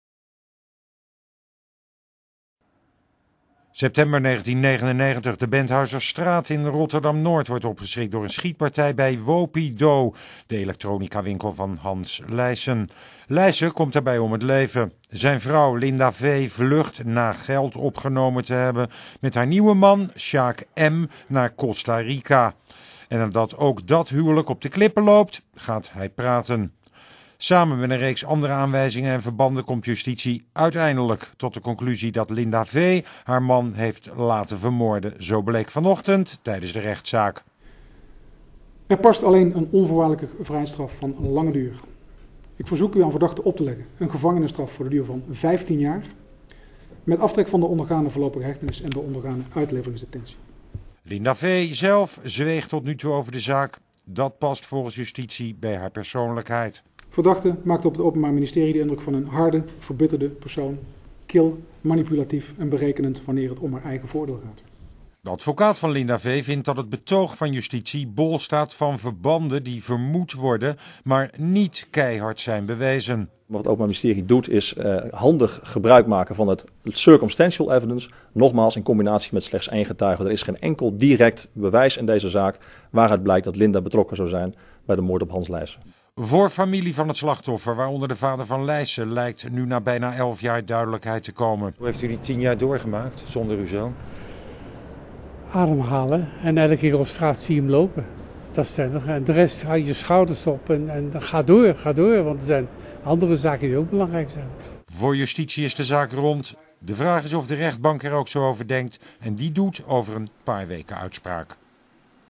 Justitie eist vijftien jaar cel wegens huurmoord
Justitie heeft vrijdag voor de Rotterdamse rechtbank vijftien jaar celstraf geëist tegen Linda V. De Rotterdamse heeft volgens het Openbaar Ministerie (OM) elf jaar jaar geleden haar man Hans Lijesen laten vermoorden.